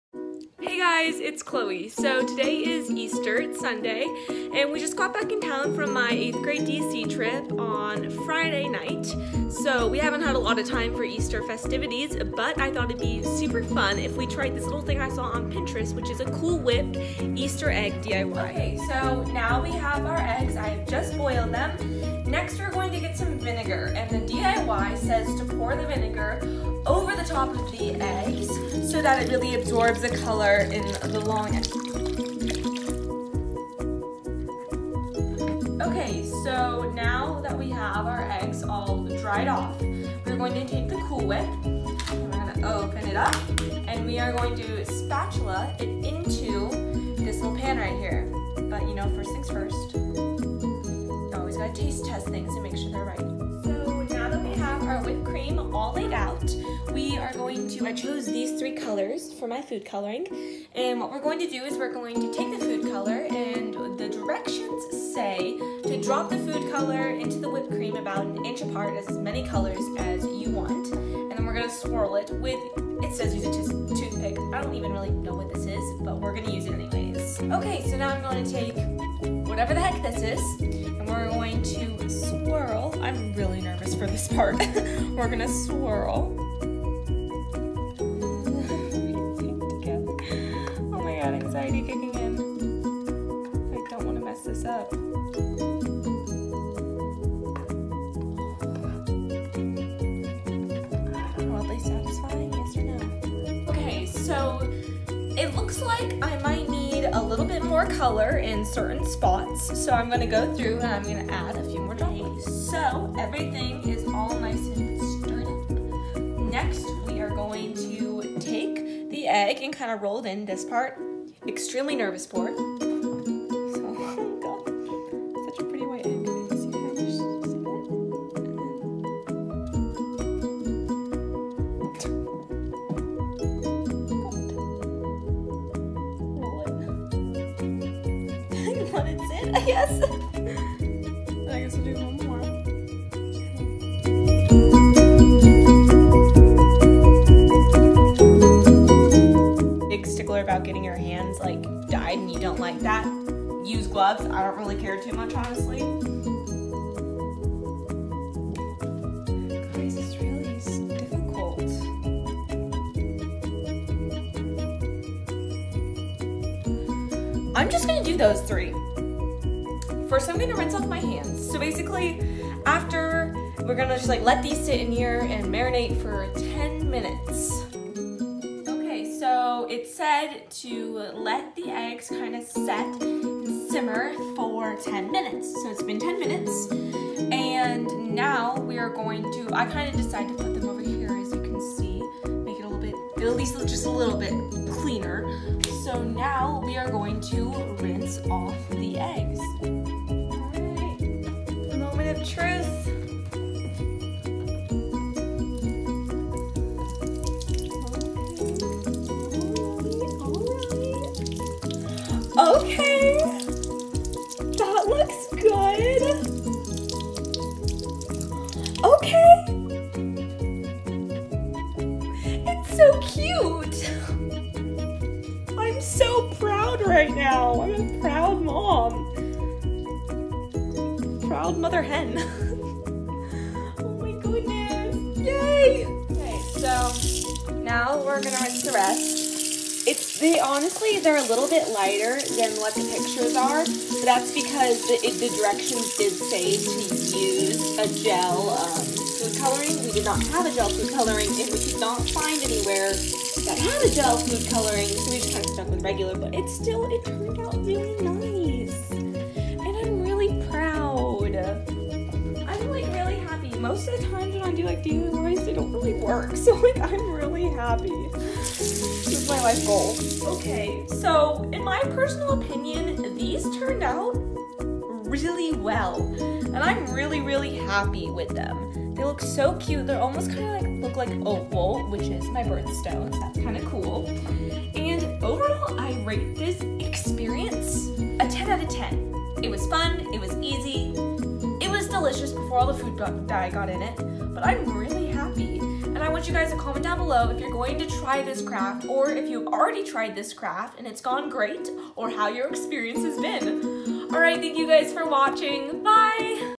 COOL WHIP MARBLE EASTER EGGS!
Check out my cool whip DIY easter eggs!! Will it work?